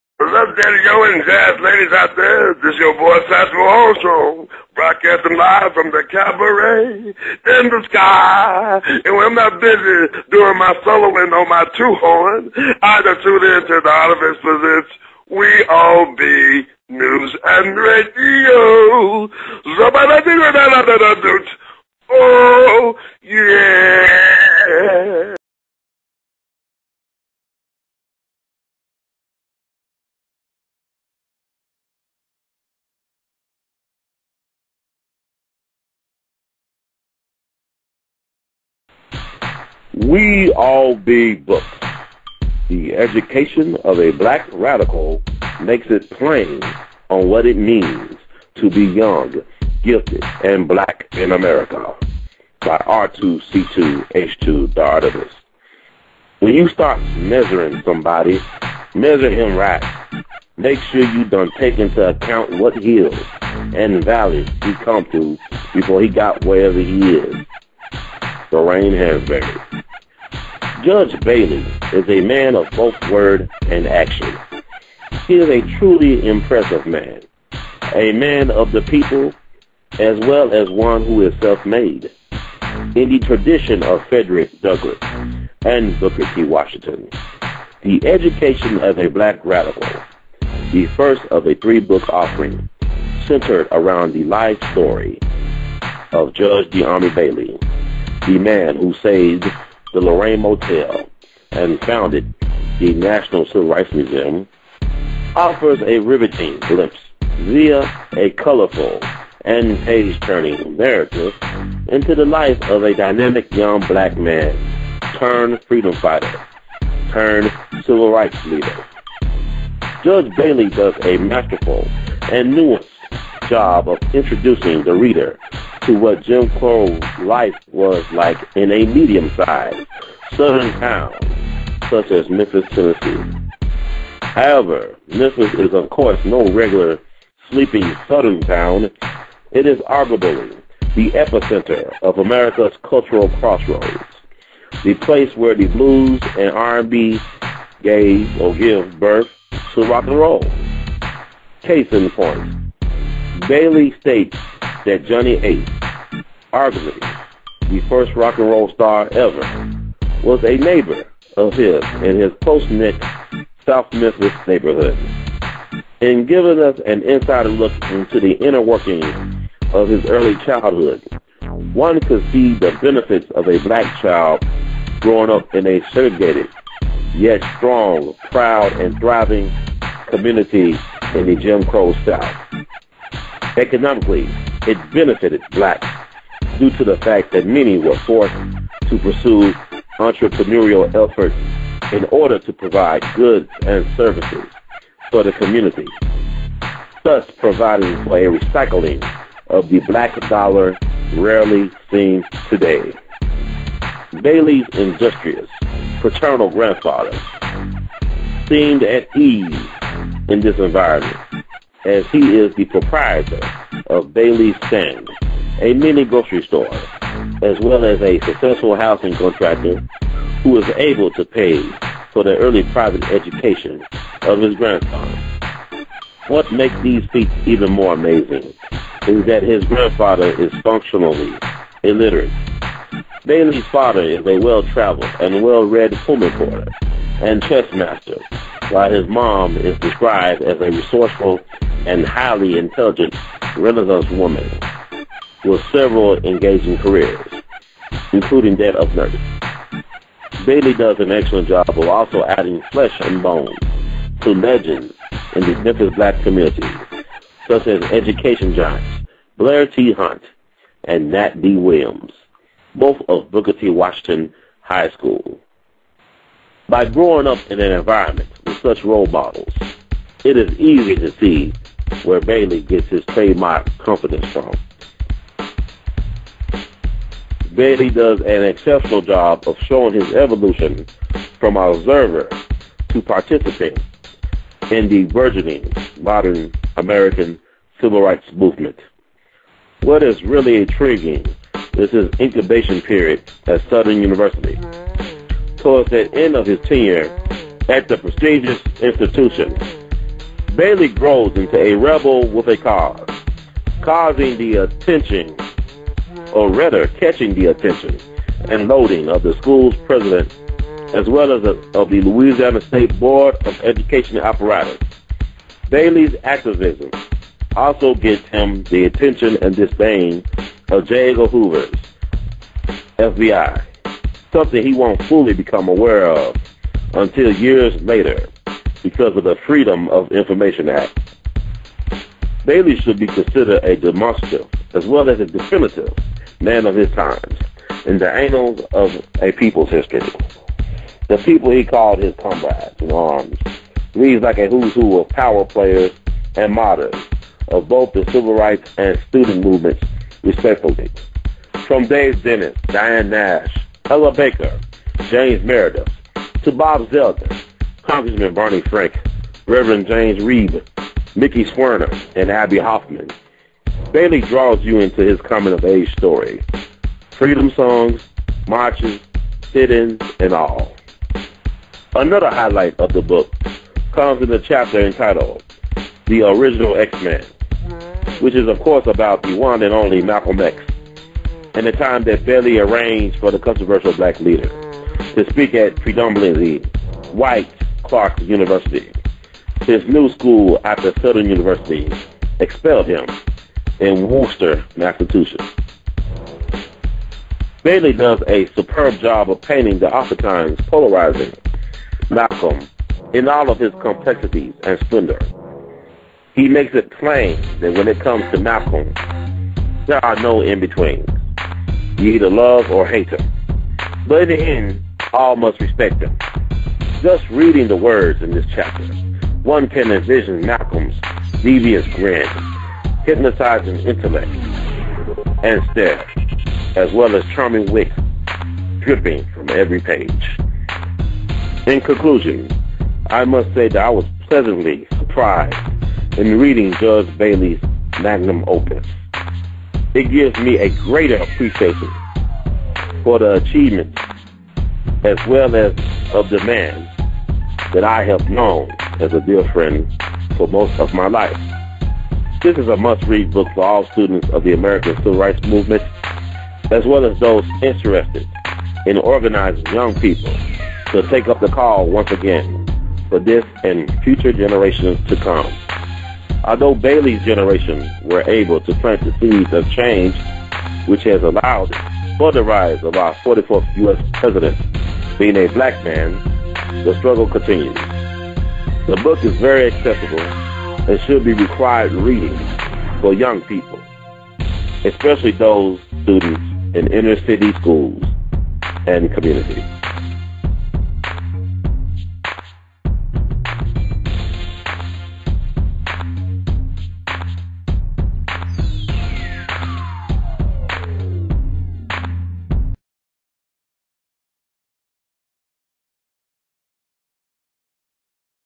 W.E. A.L.L. B.E. Books Review: The Education Of A Black Radical (Updated)
W.E. A.L.L. B.E. TV/"Real Talk With Tha Artivist" Interviews Civil Rights Legend, Co-Founder Of The National Civil Museum &amp; All Around Renaissance Man The Honorable Judge D'Army Bailey about his life and new book, The Education Of A Black Radical...

Or See It In Three Parts Via The W.E. A.L.L. B.E. TV YouTube Channel:

W.E. A.L.L. B.E. Needs Your Support...Give To Grow The Movement!